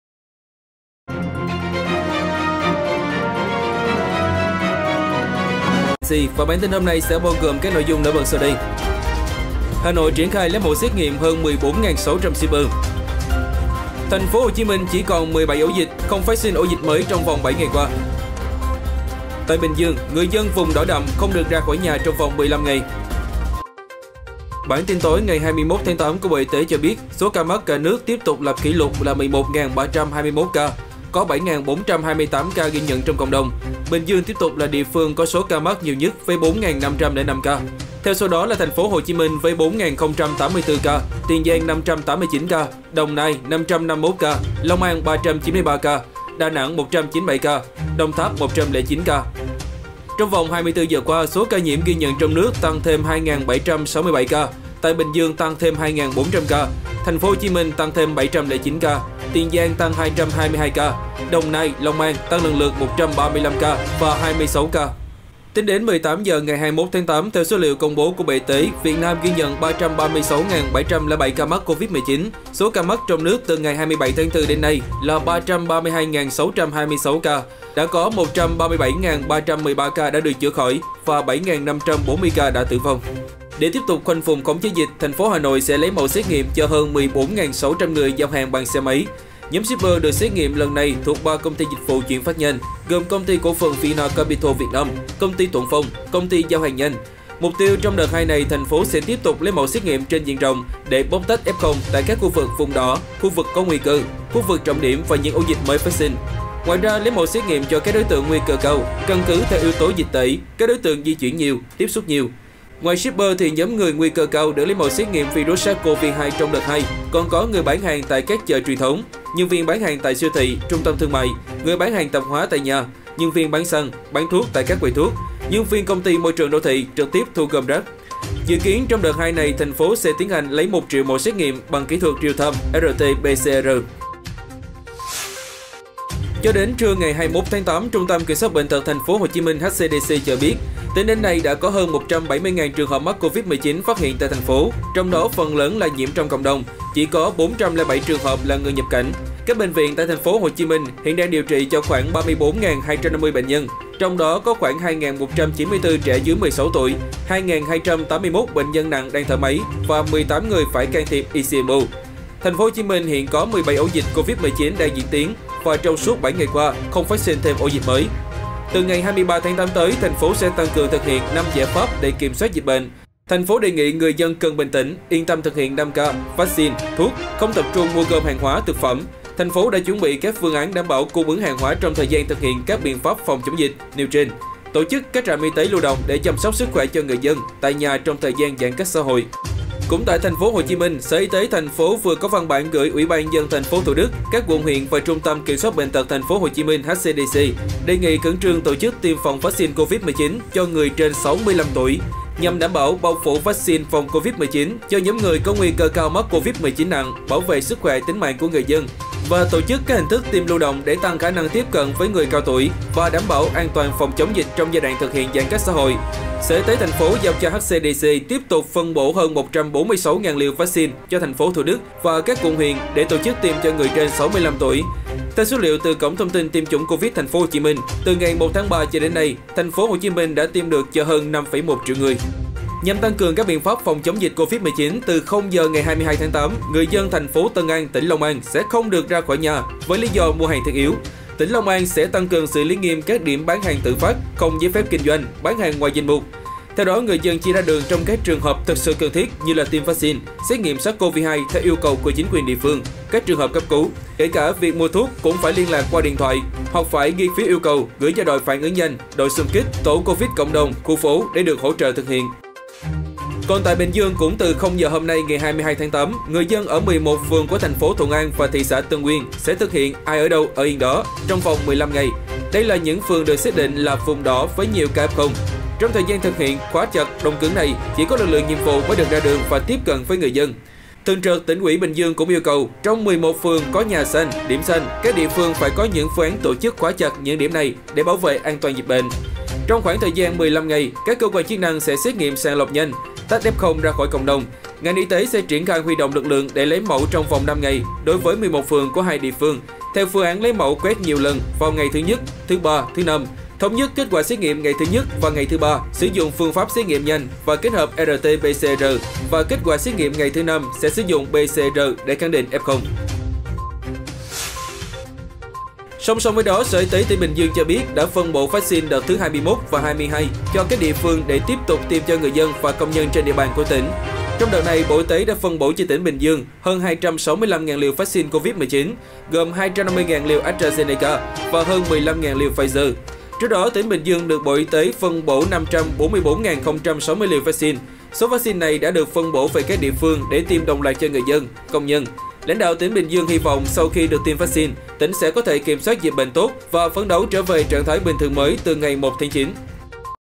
Tin tức Covid-19 mới nhất hôm nay 22/8 | Dịch virus corona việt Nam hôm nay
Thêm 11.300 ca covid -19 mới. Bình Dương tiếp tục nhiều nhất.
Hà Nội tiếp tục lấy mẫu xét nghiệm hơn 14.600 shipper.
TPHCM còn 17 ổ dịch không phát sinh ổ dịch mới.
TPHCM thực hiện tiêm vaccine mũi 1 hoàn tất cho người trên 65 tuổi.
Từ 22/8 người dân Long An không được ra ngoài mua hàng thiết yếu.